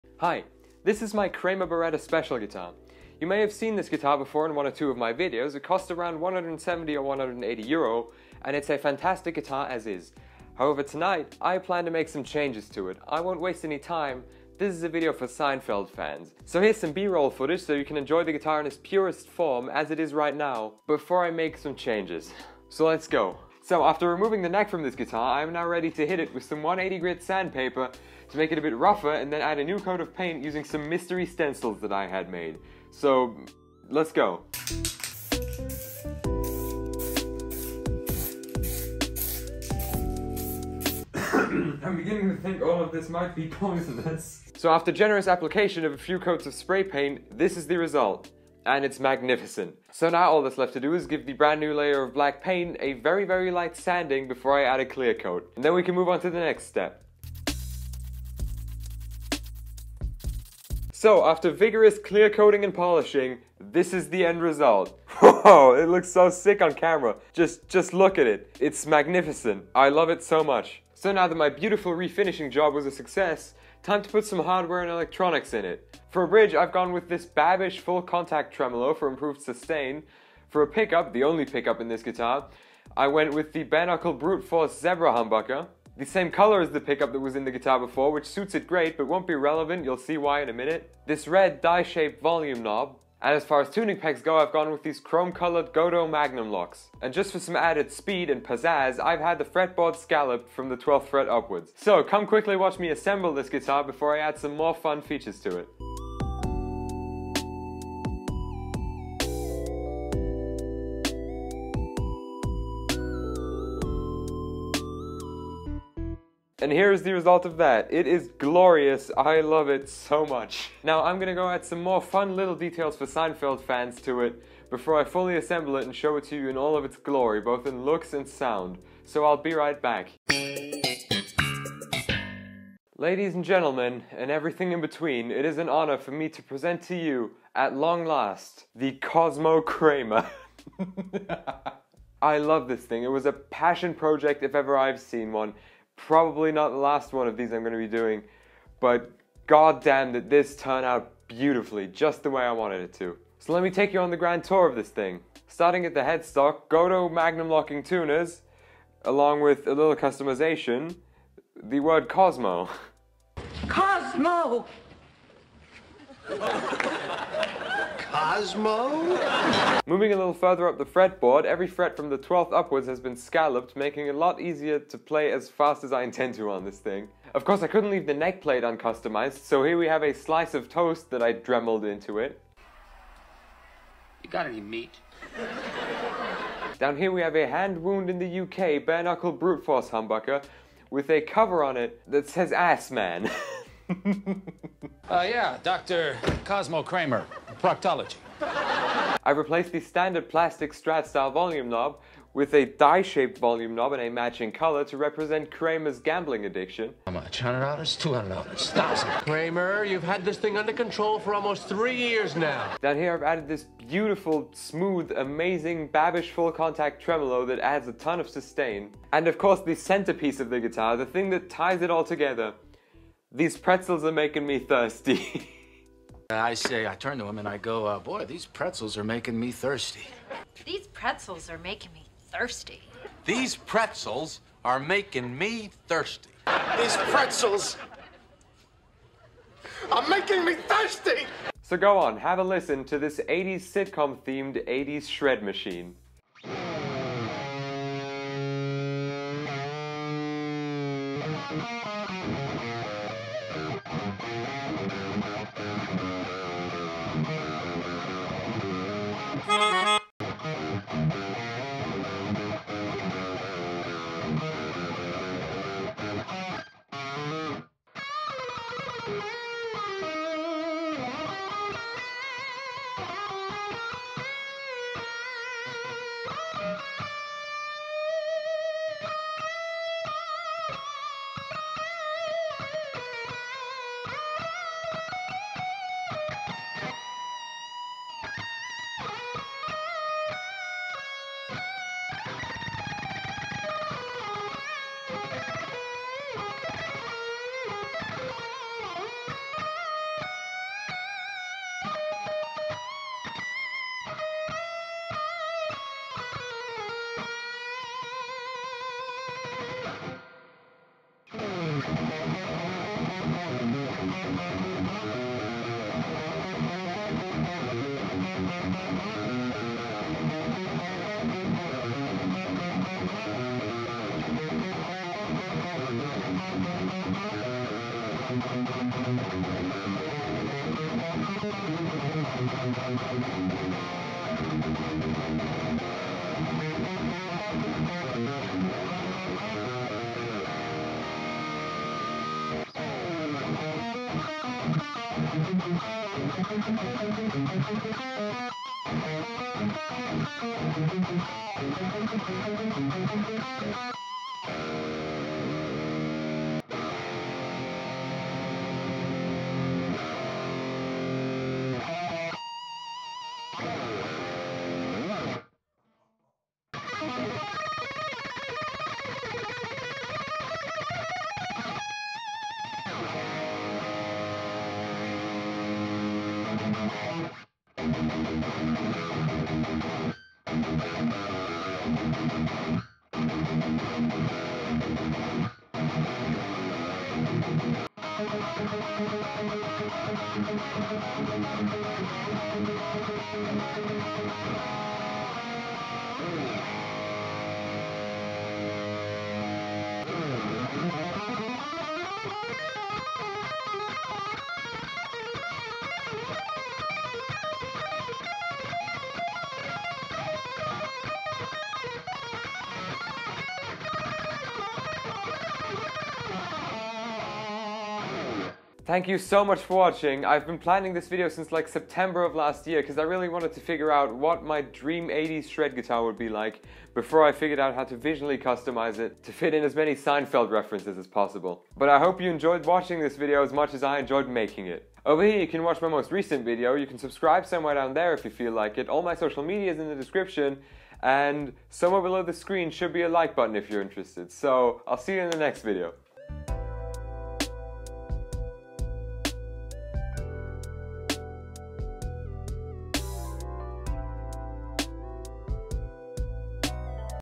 Making A Seinfeld Themed Guitar || The “Cosmo Kramer”
Tonight, I build my Seinfeld-themed 80s shred guitar. A beast I have dubbed the "Cosmo Kramer"

00:00 Build
4:02 Sound Demo
6:58 Outro